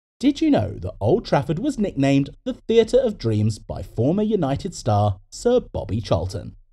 Did you know that Old Trafford ...
Did you know that Old Trafford was nicknamed the ‘Theatre of Dreams’ by former United star, Sir Bobby Charlton.